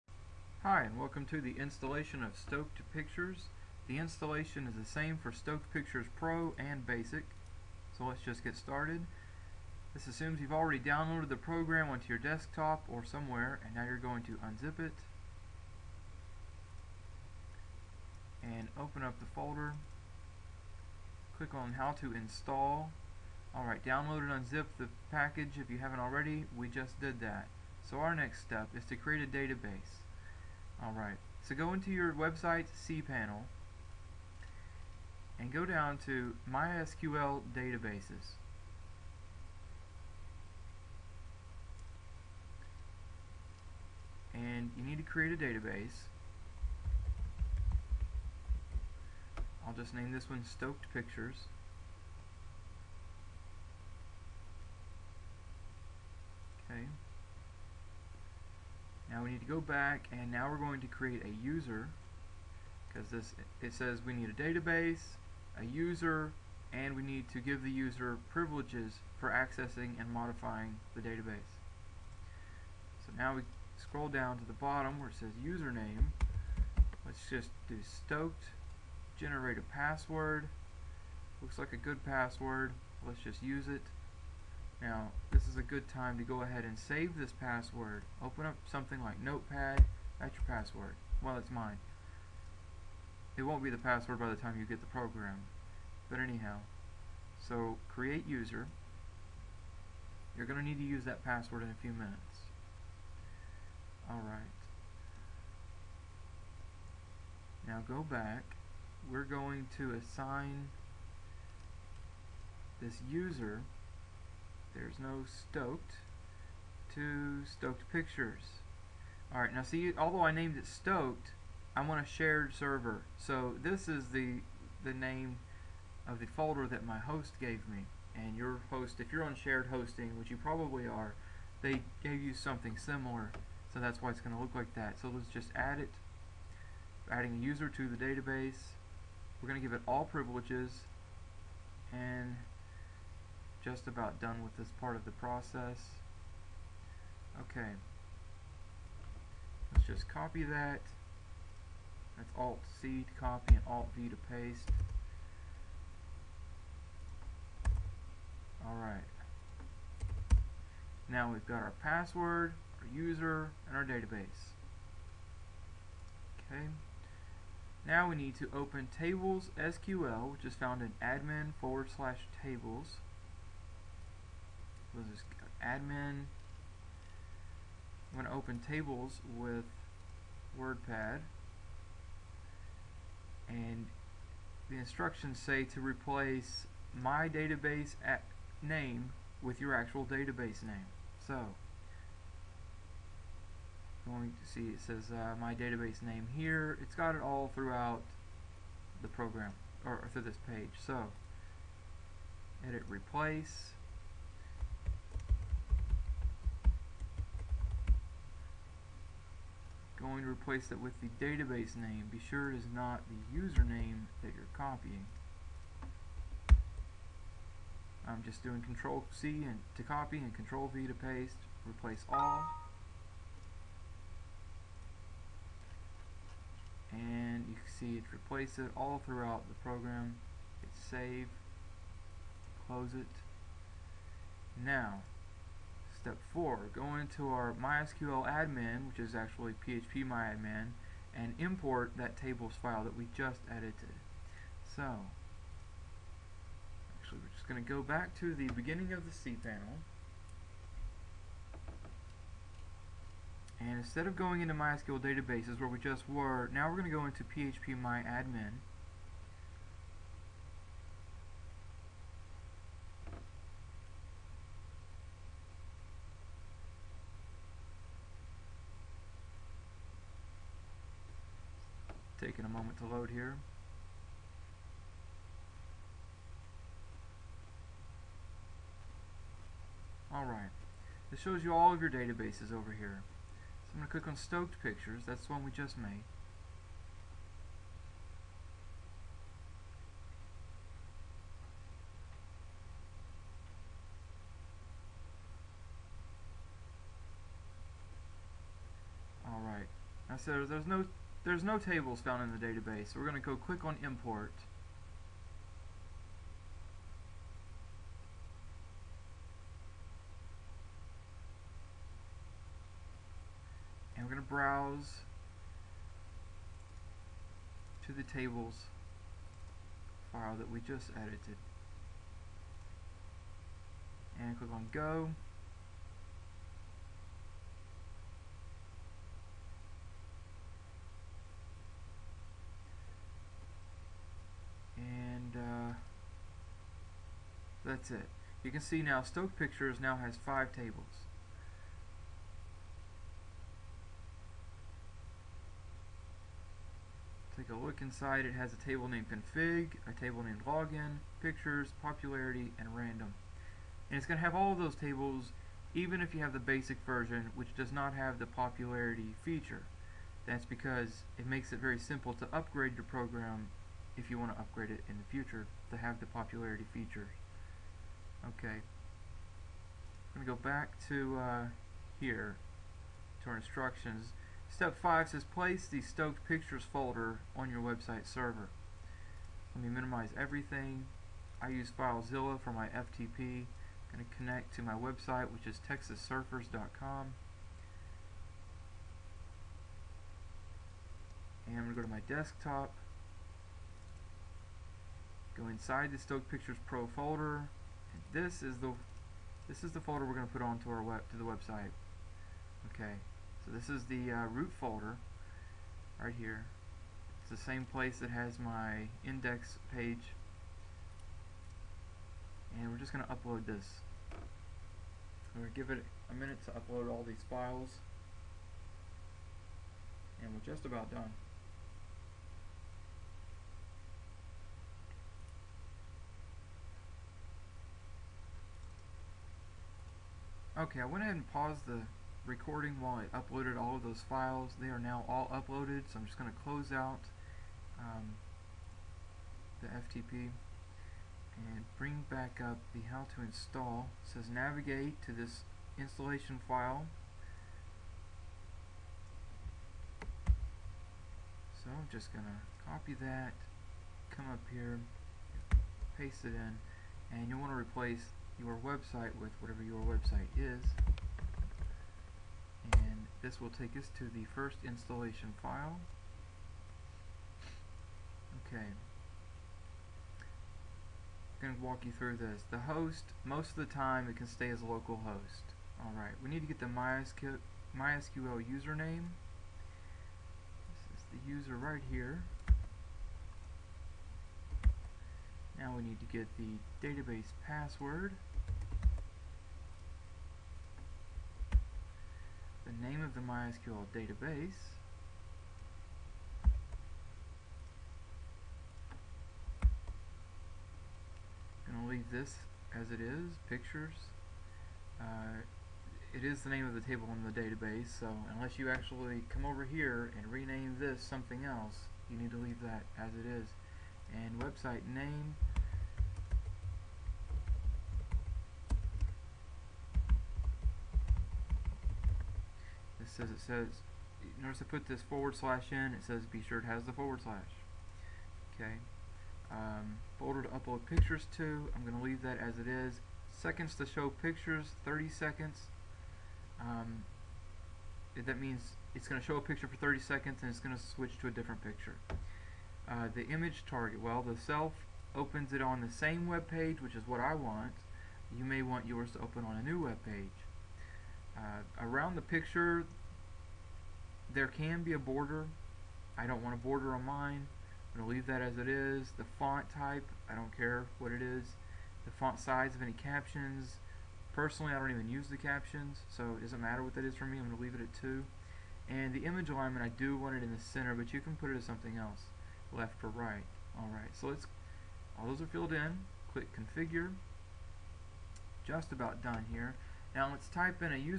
how to install stoked pictures 1
How to install Stoked Pictures, part 1.
Stoked Pictures is FREE and provides the ability to rotate pictures, schedule a picture to be the first picture shown, set a picture to always be shown first, all with an easy to use web admin. Each picture has its own alt text, caption, and link. Easily update time between pictures, border setting, and more. Requires PHP and MySQL.